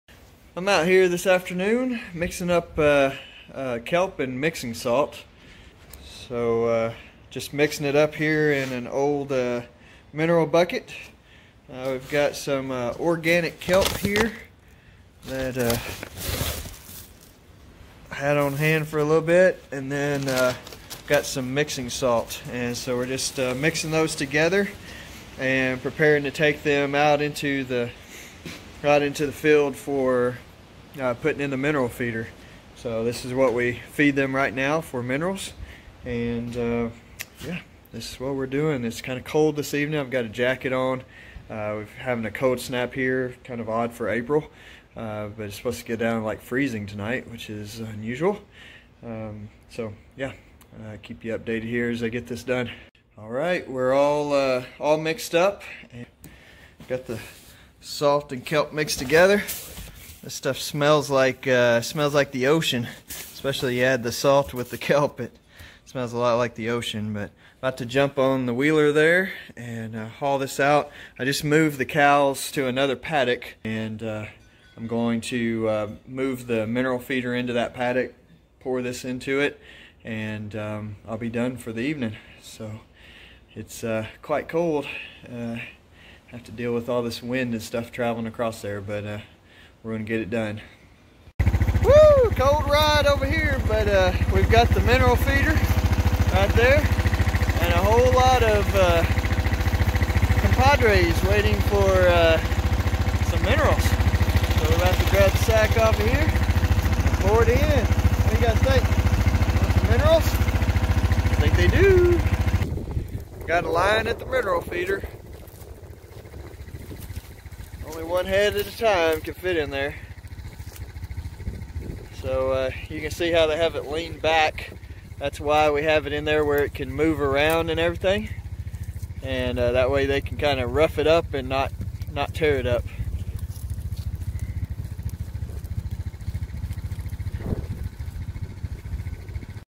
Kelp as a mineral supplement for cattle
A quick video on how we mix our kelp and feed it to cattle as a mineral supplement.

Only use organic kelp. Kelp absorbs whatever is in the water, so you want to use organic kelp.

We mix it 50/50 with mixing salt. 50lbs of kelp to 50 lbs of salt from your local feed store.